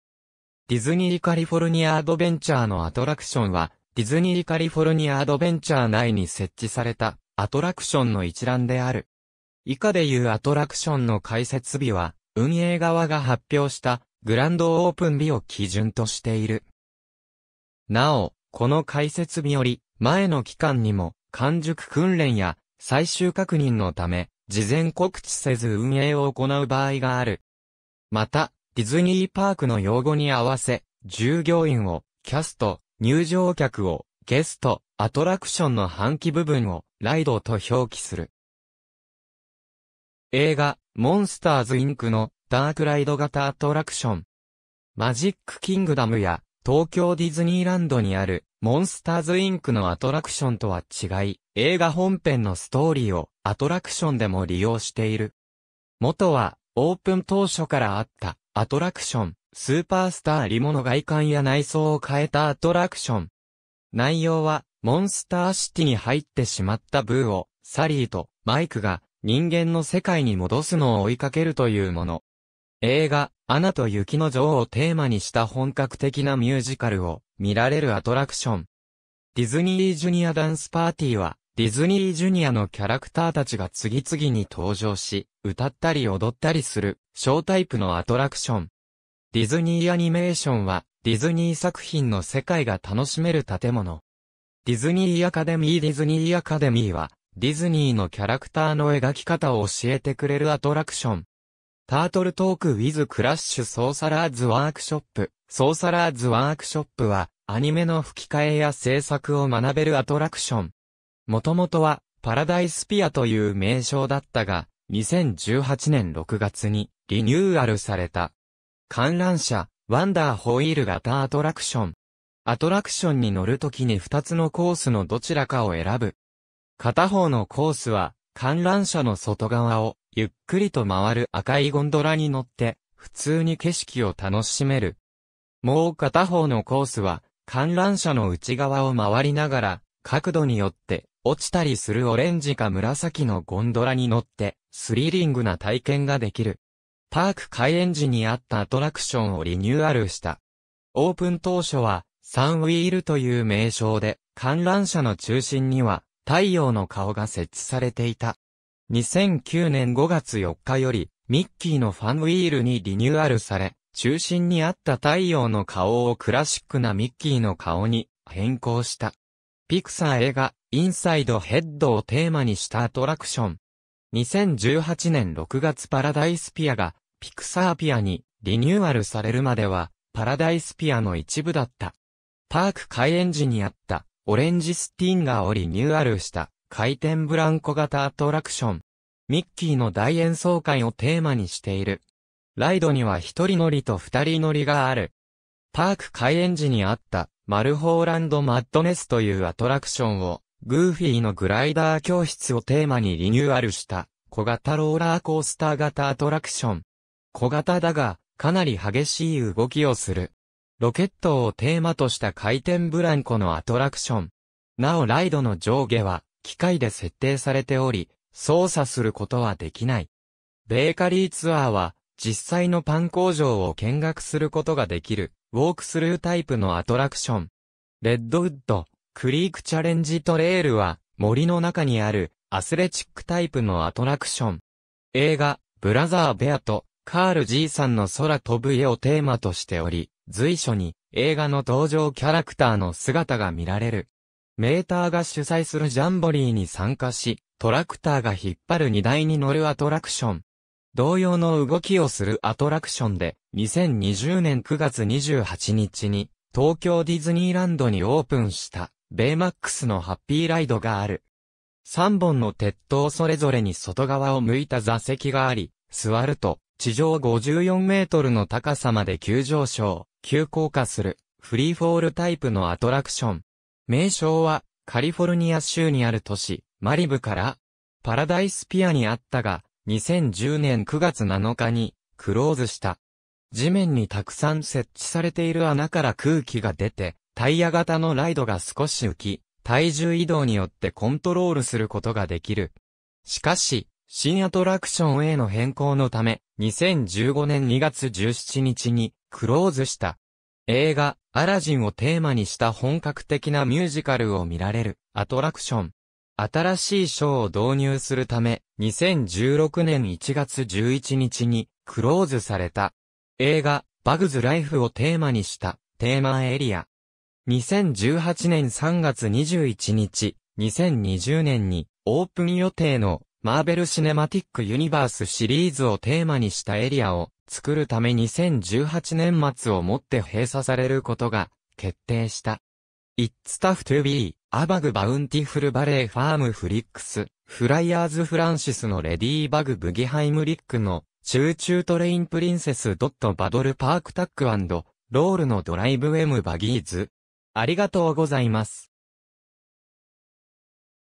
ディズニー・カリフォルニア・アドベンチャーのアトラクション
ディズニー・カリフォルニア・アドベンチャーのアトラクションはディズニー・カリフォルニア・アドベンチャー内に設置されたアトラクションの一覧である。
以下でいうアトラクションの開設日は、運営側が発表した「グランドオープン」日を基準としている。
なお、この開設日より前の期間にも、慣熟訓練や最終確認のため、事前告知せず運営を行う場合がある。
また、ディズニーパークの用語に合わせ、従業員を「キャスト」、入場客を「ゲスト」、アトラクションの搬器部分（乗り物）を「ライド」と表記する。
映画「モンスターズ・インク」のダークライド型アトラクション。
マジック・キングダムや東京ディズニーランドにあるモンスターズ・インクのアトラクションとは違い、映画本編のストーリーをアトラクションでも利用している。
元は、オープン当初からあったアトラクション「スーパースター・リモ」の外観や内装を変えたアトラクション。
内容は、モンスターシティに入ってしまったブーを、サリーとマイクが人間の世界に戻すのを追いかけるというもの。
映画「アナと雪の女王」をテーマにした本格的なミュージカルを見られるアトラクション。
ディズニー・ジュニア・ダンス・パーティー(Disney Junior Dance Party)は、ディズニー・ジュニアのキャラクター達が次々に登場し、歌ったり踊ったりするショータイプのアトラクション。
ディズニー・アニメーション(Disney Animation)は、ディズニー作品の世界が楽しめる建物。
ディズニー・アカデミー ディズニー・アカデミー(Animation Academy)は、ディズニーのキャラクターの描き方を教えてくれるアトラクション。
タートル・トーク・ウィズ・クラッシュ ソーサラーズ・ワークショップ ソーサラーズ・ワークショップ(Sorcerer's Workshop)は、アニメの吹き替えや制作を学べるアトラクション。
元々は「パラダイス・ピア」という名称だったが、2018年6月にリニューアルされた。
観覧車「ワンダー・ホイール」型アトラクション。
アトラクションに乗る時に2つのコースのどちらかを選ぶ。
片方のコースは、観覧車の外側をゆっくりと回る赤いゴンドラに乗って、普通に景色を楽しめる。
もう片方のコースは、観覧車の内側を回りながら角度によって落ちたりするオレンジか紫のゴンドラに乗って、スリリングな体験ができる。
パーク開園時にあったアトラクションをリニューアルした。
オープン当初は「サンウィール」という名称で、観覧車の中心には太陽の顔が設置されていた。
2009年5月4日より「ミッキーのファンウィール」にリニューアルされ、中心にあった太陽の顔をクラシックなミッキーの顔に変更した。
ピクサー映画「インサイド・ヘッド」をテーマにしたアトラクション。
2018年6月パラダイス・ピアがピクサー・ピアにリニューアルされるまでは、パラダイス・ピアの一部だった。
パーク開園時にあった「オレンジ・スティンガー」をリニューアルした回転ブランコ型アトラクション。
「ミッキーの大演奏会」をテーマにしている。
ライドには1人乗りと2人乗りがある。
パーク開園時にあった「マルホーランド・マッドネス」というアトラクションを「グーフィーのグライダー教室」をテーマにリニューアルした小型ローラーコースター型アトラクション。
小型だが、かなり激しい動きをする。
ロケットをテーマとした回転ブランコのアトラクション。
なおライドの上下は機械で設定されており、操作することは出来ない。
ベーカリー・ツアー(The Bakery Tour)は、実際のパン工場を見学することが出来るウォークスルータイプのアトラクション。
レッドウッド・クリーク・チャレンジ・トレイル(Redwood Creek Challenge Trail)は、森の中にあるアスレチックタイプのアトラクション。
映画『ブラザー・ベア』と『カールじいさんの空飛ぶ家』をテーマとしており、随所に映画の登場キャラクターの姿が見られる。
メーターが主催するジャンボリーに参加し、トラクターが引っ張る荷台に乗るアトラクション。
同様の動きをするアトラクションで、2020年9月28日に東京ディズニーランドにオープンした「ベイマックスのハッピーライド」がある。
3本の鉄塔それぞれに外側を向いた座席があり、座ると地上54mの高さまで急上昇、急降下するフリーフォールタイプのア...